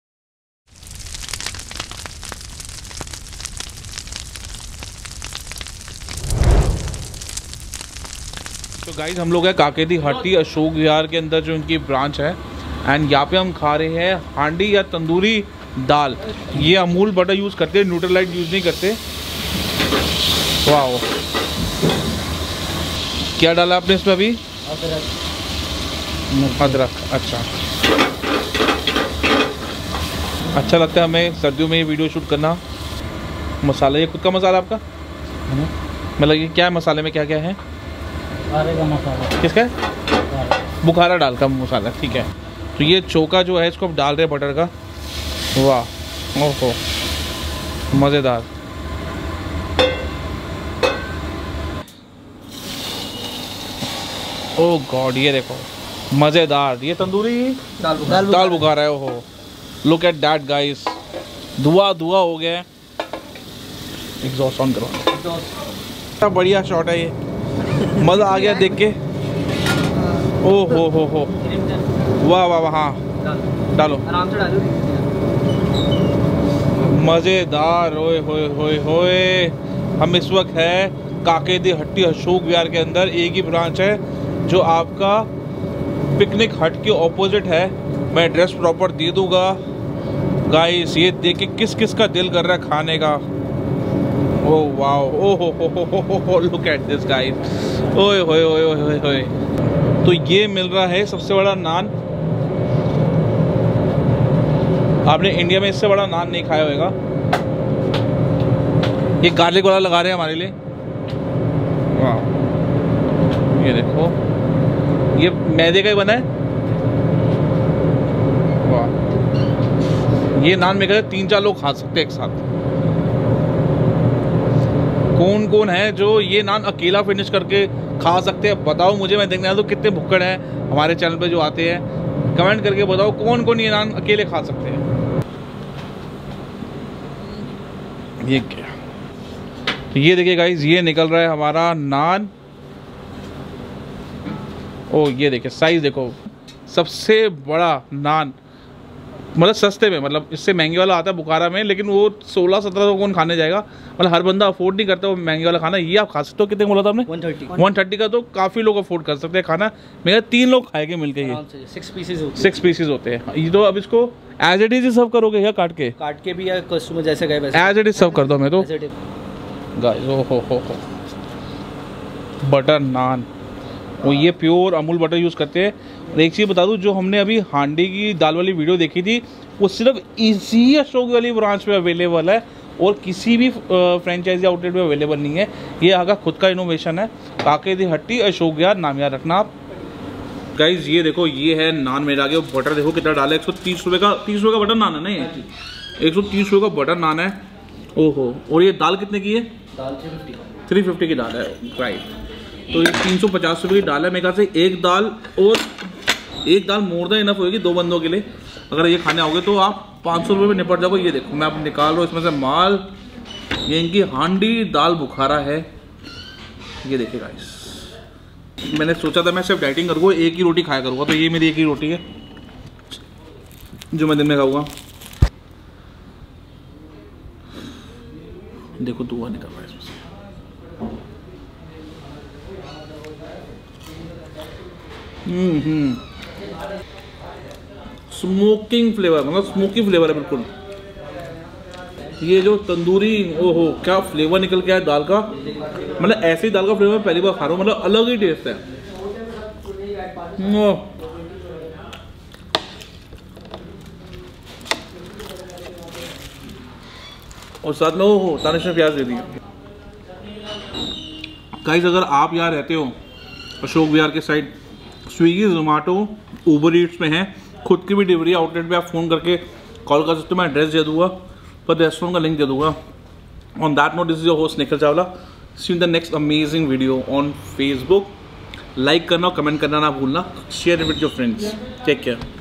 Tandoori Dal Bhukara at kake di hatti Ashok Vihar | Vegetarian Food
kake di hatti is an icon in serving vegetarian food in old Delhi. The Franchise in Ashok Vihar has taken their Daal to the next level. The masala which they add to this daal is only available at this outlet only and not any other outlet as of the date